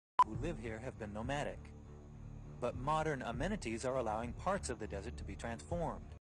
International Film & Video
A haze lays over the desert of Mt. Sinai.